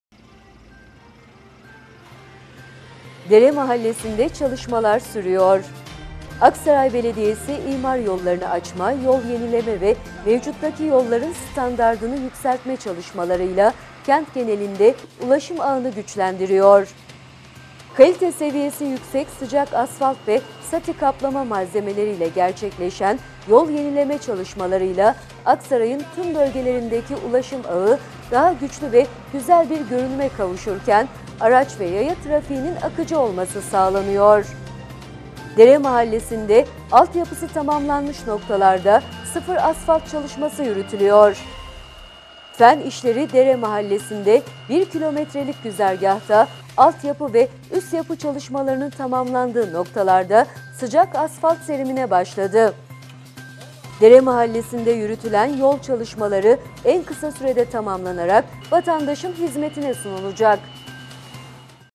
DERE MAHALLESİ’NDE ÇALIŞMALAR SÜRÜYOR
AKSARAY BELEDİYESİ, İMAR YOLLARINI AÇMA, YOL YENİLEME VE MEVCUTTAKİ YOLLARIN STANDARDINI YÜKSELTME ÇALIŞMALARIYLA KENT GENELİNDE ULAŞIM AĞINI GÜÇLENDİRİYOR.
KALİTE SEVİYESİ YÜKSEK SICAK ASFALT VE SATHİ KAPLAMA MALZEMELERİYLE GERÇEKLEŞEN YOL YENİLEME ÇALIŞMALARIYLA AKSARAY’IN TÜM BÖLGELERİNDEKİ ULAŞIM AĞI DAHA GÜÇLÜ VE GÜZEL BİR GÖRÜNÜME KAVUŞURKEN, ARAÇ VE YAYA TRAFİĞİNİN AKICI OLMASI SAĞLANIYOR.
DERE MAHALLESİ’NDE ALTYAPISI TAMAMLANMIŞ NOKTALARDA SIFIR ASFALT ÇALIŞMASI YÜRÜTÜLÜYOR.
FEN İŞLERİ, DERE MAHALLESİ’NDE  1 KİLOMETRELİK GÜZERGAHTA ALTYAPI VE ÜSTYAPI ÇALIŞMALARININ TAMAMLANDIĞI NOKTALARDA SICAK ASFALT SERİMİNE BAŞLADI.
DERE MAHALLESİ’NDE YÜRÜTÜLEN YOL ÇALIŞMALARI EN KISA SÜREDE TAMAMLANARAK VATANDAŞIN HİZMETİNE SUNULACAK.